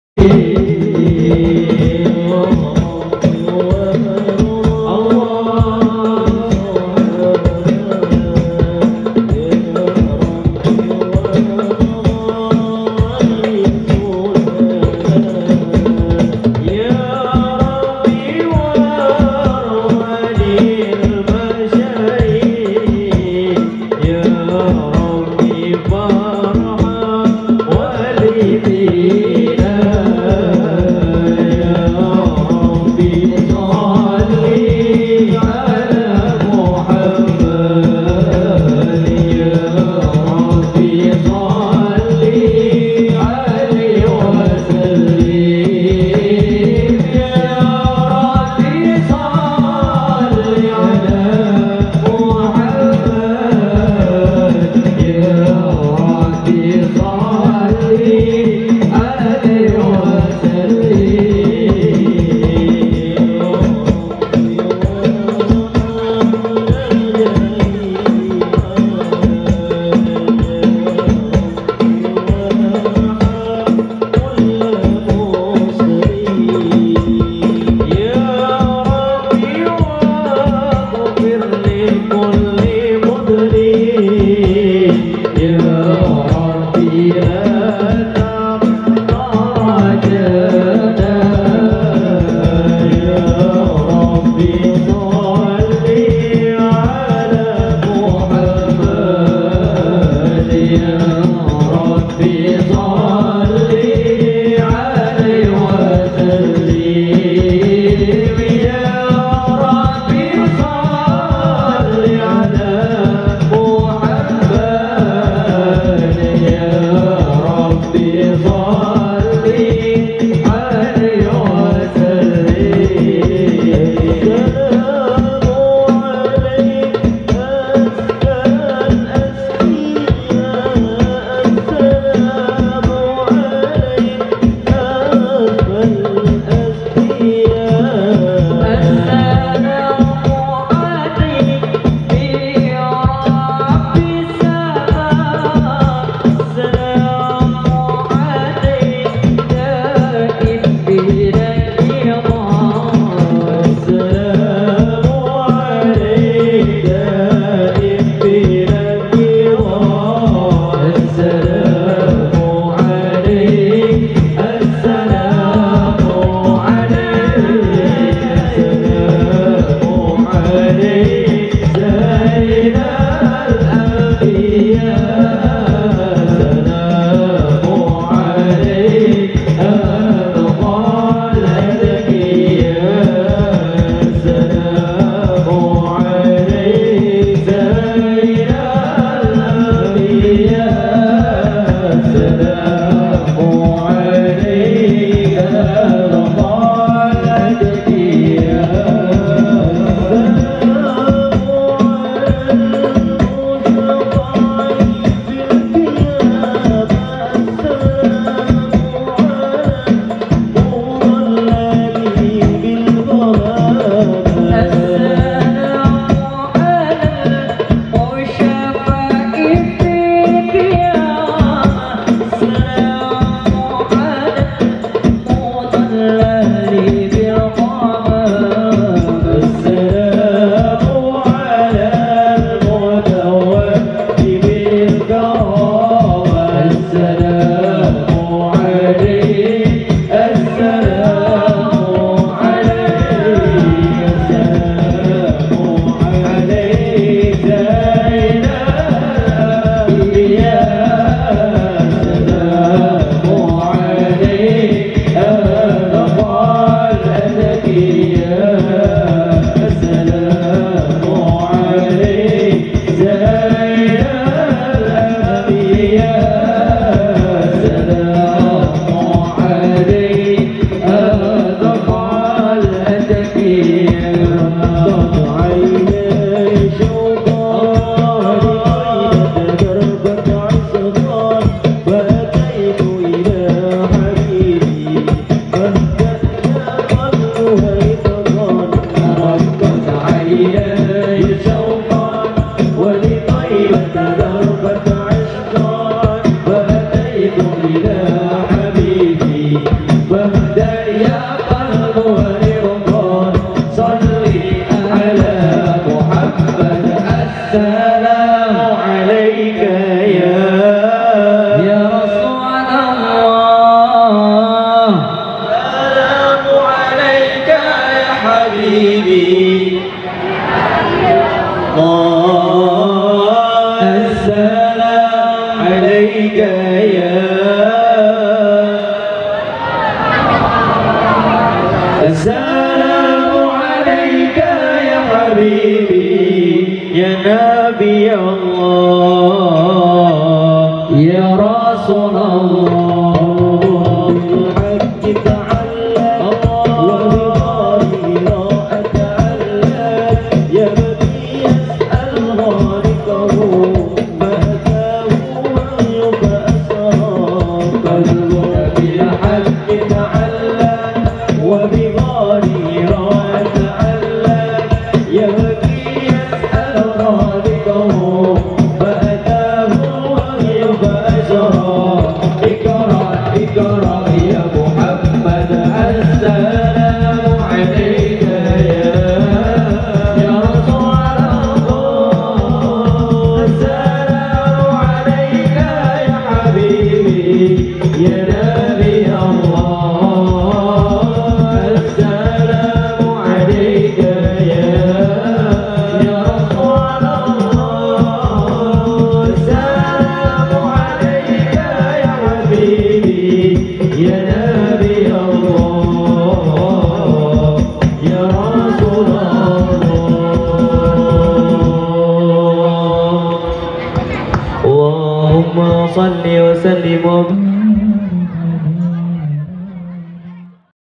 AL MALIKI LIVE SARWODADI FEAT WAHYU AJI
JAMA'AH MAULID SIMTUDDUROR
AL MALIKI PECANGAKAN
COMAL PEMALANG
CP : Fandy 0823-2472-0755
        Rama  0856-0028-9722